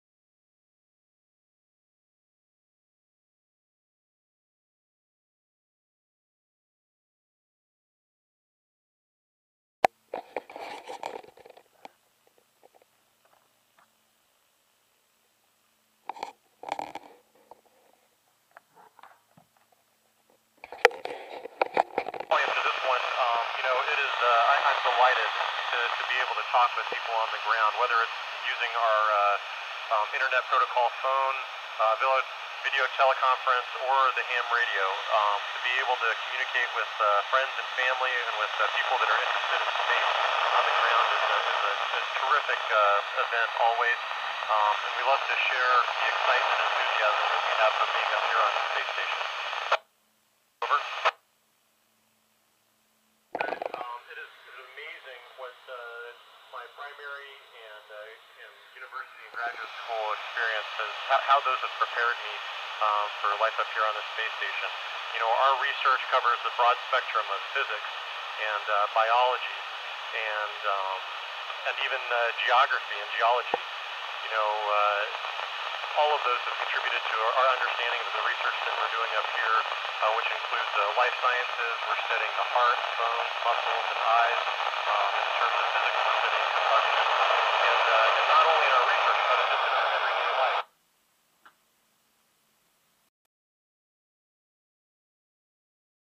Listening to Astronauts!
This sounds to me like astronaut Scott Kelly talking to HAM radio operators while flying 250 miles or so above the UK. This was around 12:00 noon on 145.800MHz. Great stuff!